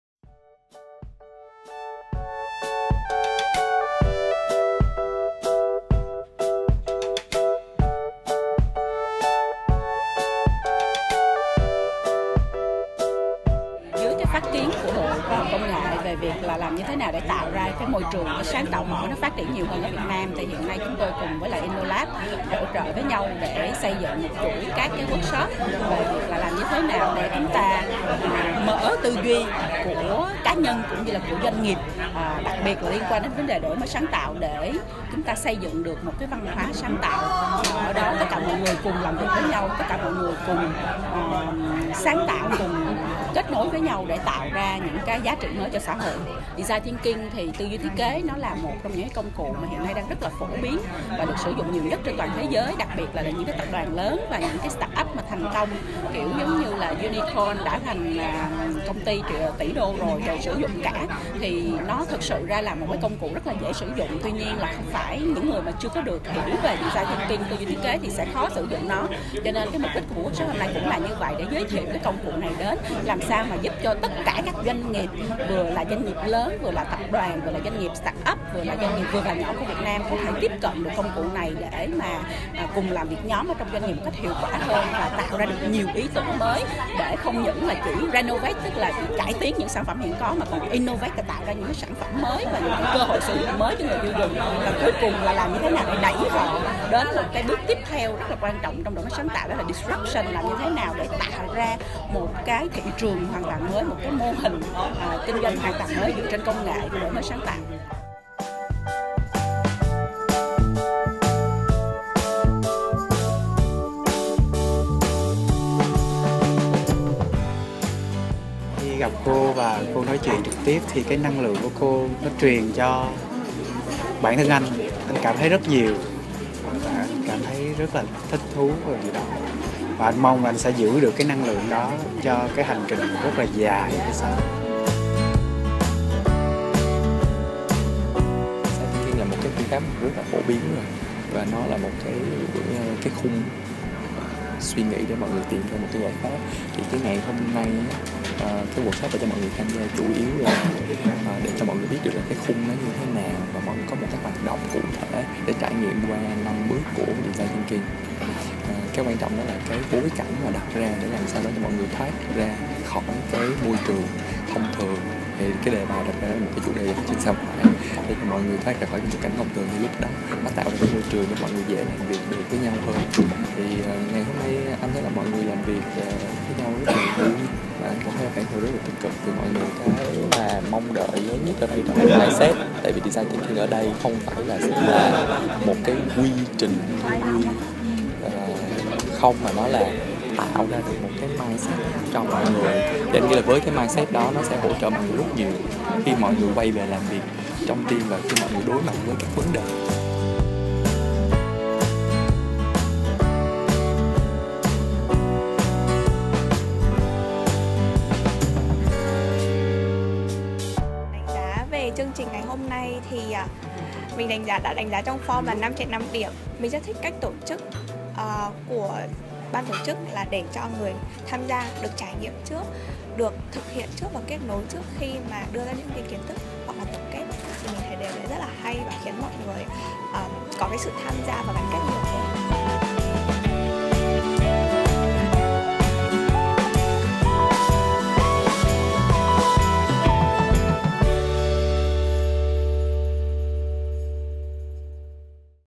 ĐỔI MỚI SÁNG TẠO BẰNG TƯ DUY THIẾT KẾ | NGUYỄN PHI VÂN | INNOLAB WORKSHOP
FIRST INNOVATION THROUGH DESIGN THINKING WORKSHOP

“Innovation is about what you don’t know” – Đổi mới sáng tạo là những gì bạn không biết.

Vậy với những điều mình không biết, trở ngại nào lớn nhất khiến chúng ta chùn bước trước sáng tạo, đó chính là BẢN THÂN CHÚNG TA.

Sáng tạo không phải là hành trình để học mà là hành trình buông bỏ những gì mình đã học, đã biết. Vì khi đã cho rằng mình biết quá nhiều, chúng ta có xu hướng cố chấp tiếp nhận những cái mới, và đó chính là rào cản lớn nhất của sáng tạo bên trong mỗi con người.

Buổi đào tạo về Tư duy sáng tạo đổi mới bằng công cụ Tư duy thiết kế bằng phương pháp Học qua Chơi được InnoLab Asia và Arkki ASEAN tổ chức vào ngày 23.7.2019.